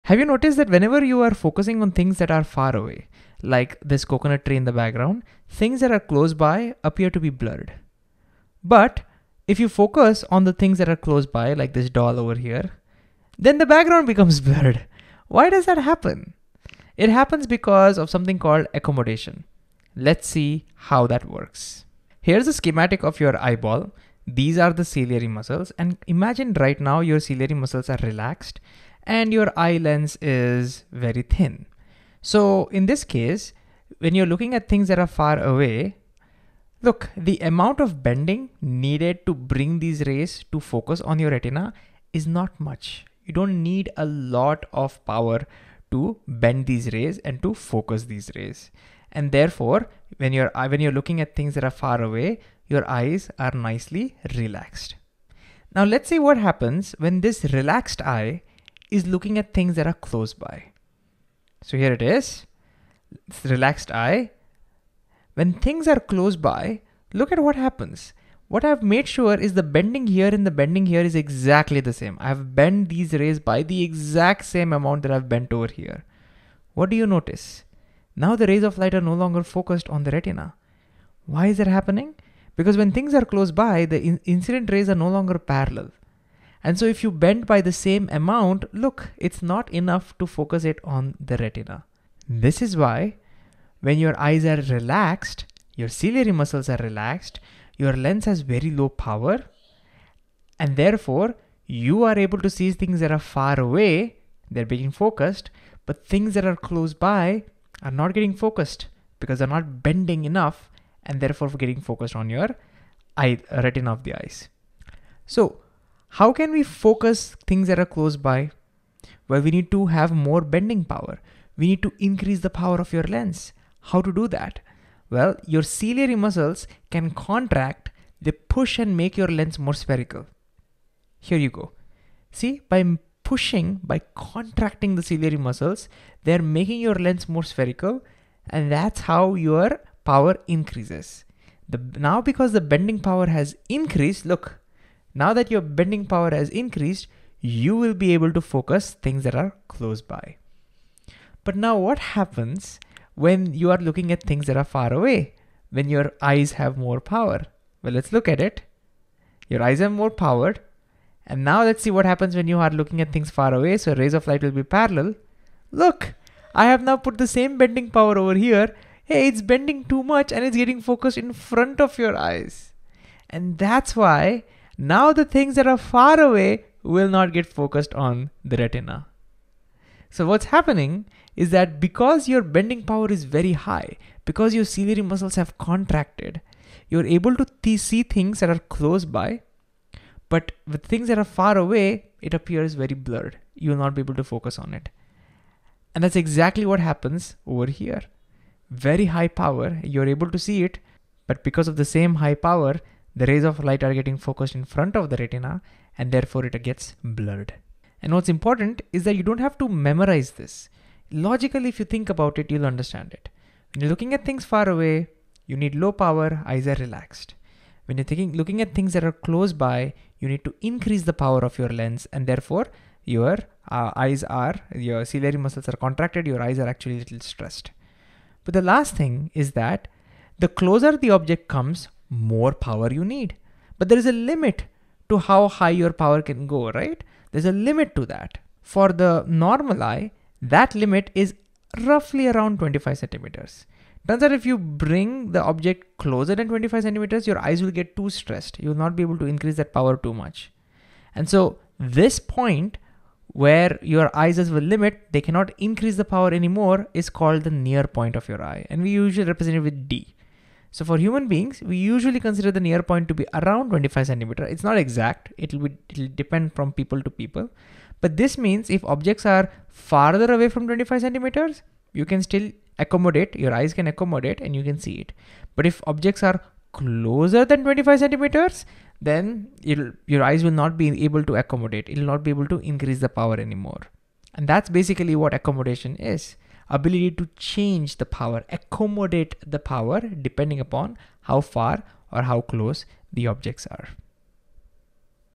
Power of accommodation | The human eye and the colourful world | physics class 10 | Khan Academy
Let's explore how accommodation helps our eyes focus on various things